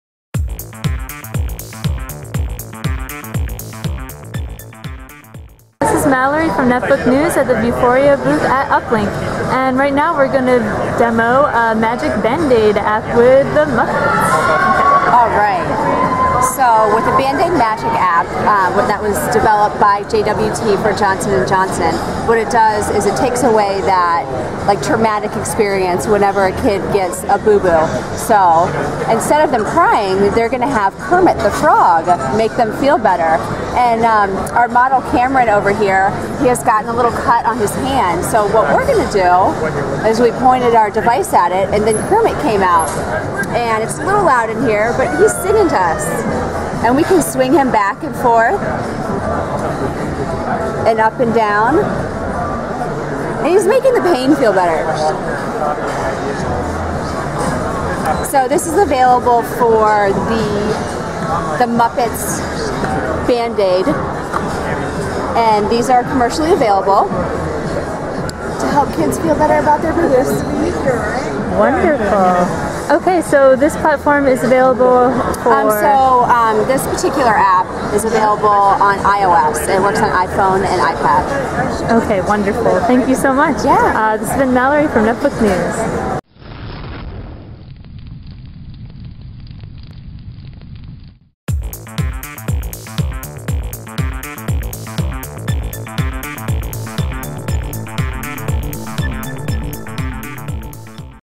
Muppets Come to Life with Band-Aid Magic Vision App - Uplinq 2012
Band-Aid Magic Vision App - At Uplinq we got a hands-on video demo of the Band-Aid Magic Vision App frpm Qualcomm's Vuforia. The app features the Muppets and is available for iOS.